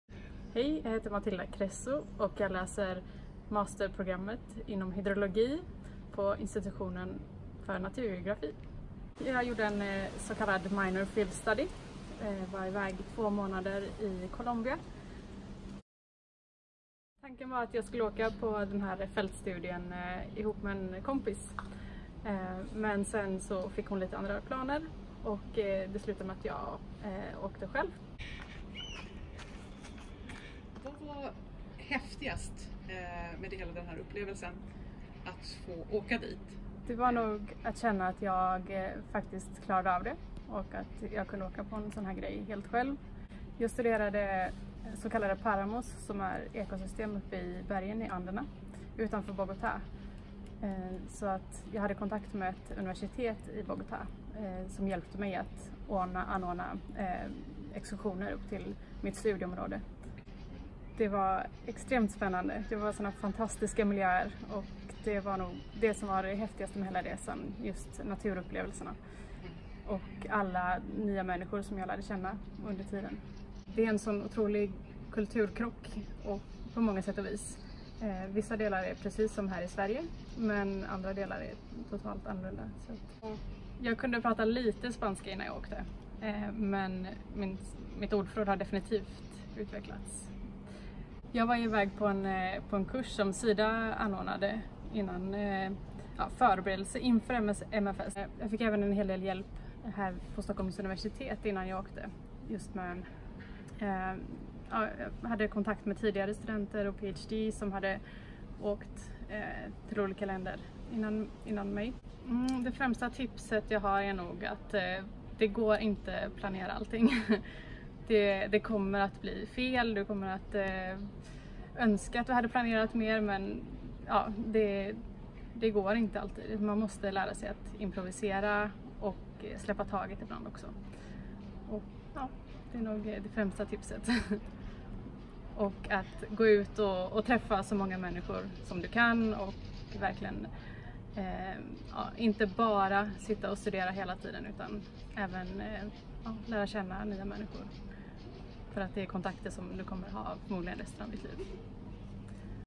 Matilda Cresso, masterstudent i hydrologi, berättar om sin Minor field study (MFS) i Colombia.
Matilda Cresso, masterstudent på programmet hydrologi, hydrogeologi och vattenresurser på Institutionen för naturgeografi, Stockholms universitet, berättar om när hon resta iväg på en Minor field study (MFS) till Colombia.